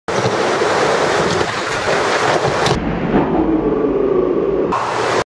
SLAMM at Nangang Bowl
Just learned back smith grinds on ramp but. the ramp gat wet and I SLAMMED!!